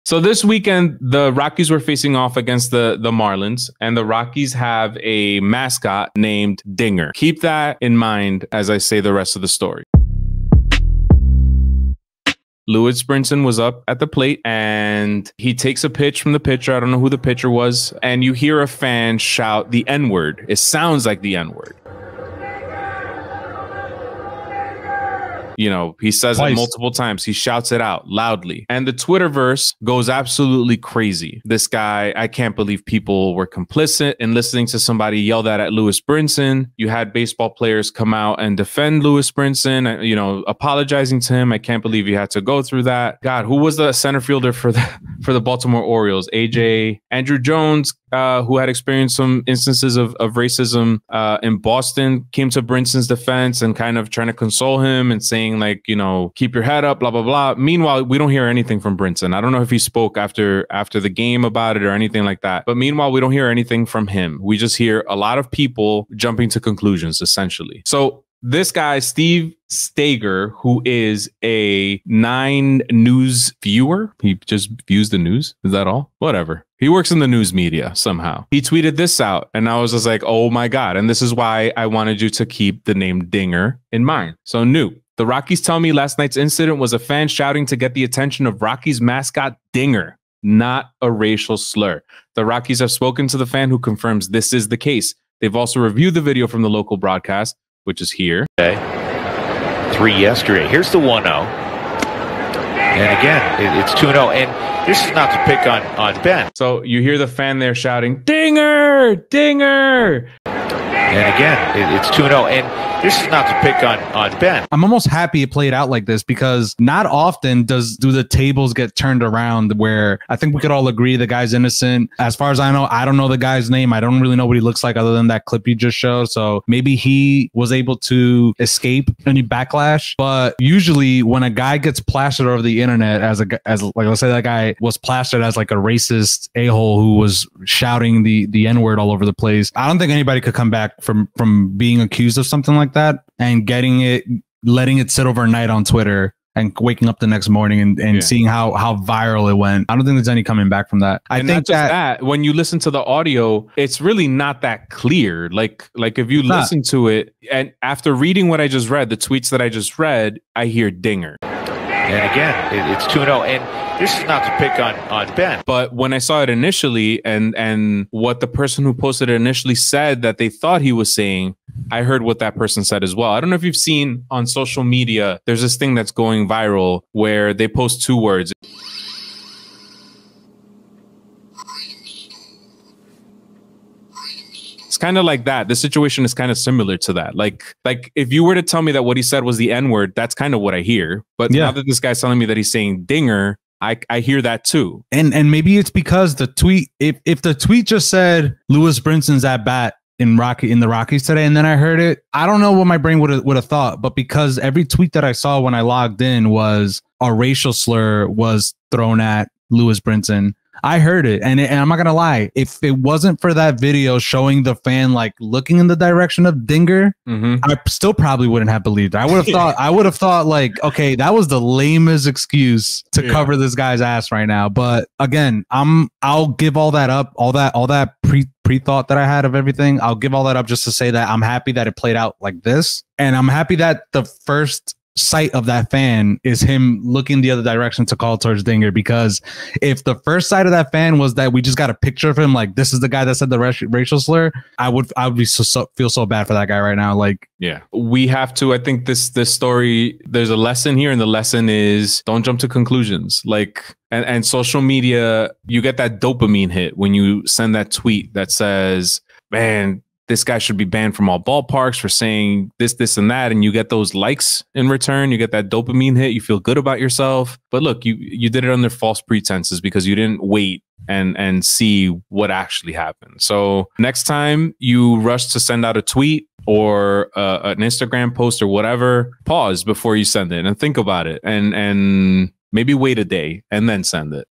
Dinger Video | Colorado Rockies Racial Slur
Today in Bodega Baseball, we discuss the details surrounding the infamous Dinger video.

Last weekend, a video surfaced that appeared to show a Colorado Rockies fan using a racial slur during a Lewis Brinson at-bat. As it turns out, this wasn't an example of racism playing out in public. The fan in question was not shouting the n-word, he was calling for Rockies mascot, Dinger.

Thankfully, this was all cleared up by 9News weekend anchor Steve Staeger on twitter

Follow the hosts on Twitter!